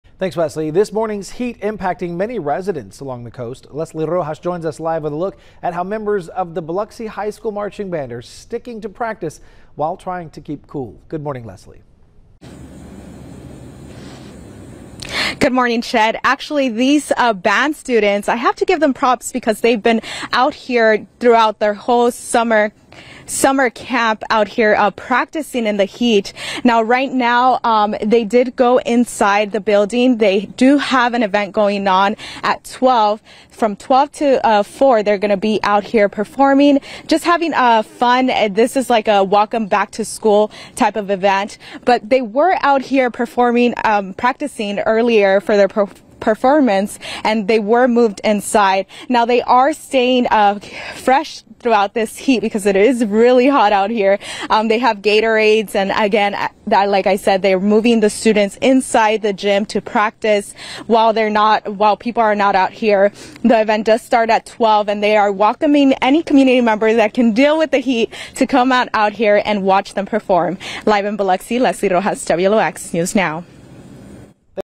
LIVE: Biloxi HS Band previews upcoming show
The Biloxi High School band held a community event Friday afternoon, meant to preview part of this season's halftime show and get students excited for the start